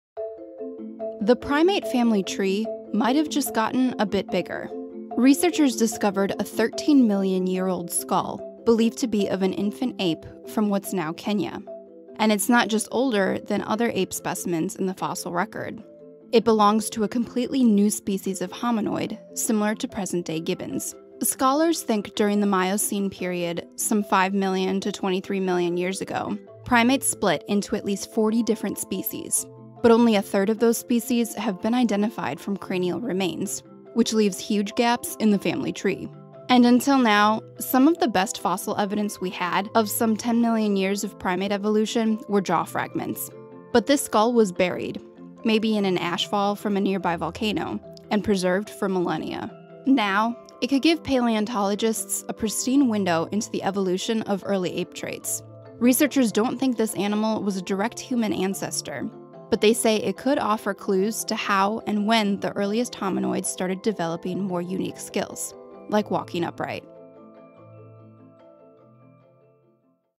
13 million year old skull may fill evolution gap
A newly discovered fossil appears to belong to an entirely new primate species, researchers say.